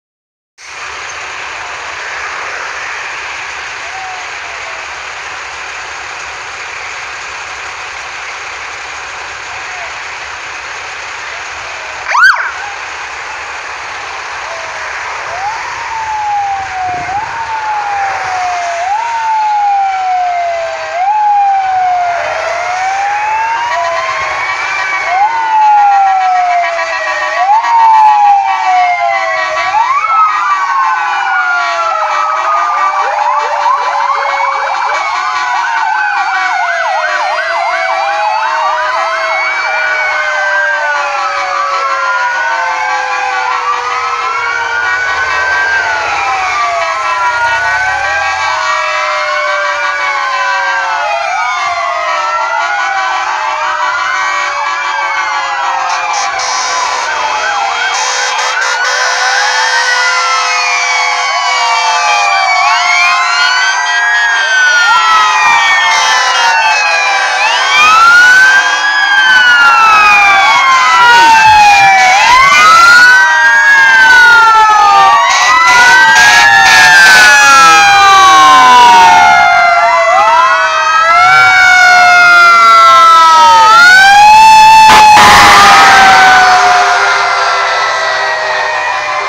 Cortejo Funebre del Mártir HERNAN AVILES (Q.E.P.D)
Bomberos rinde honores en el km 140 San Fernando al paso del cortejo funebre del martir de bomberos de Chile HERNAN.AVILES (Q.E.P.D)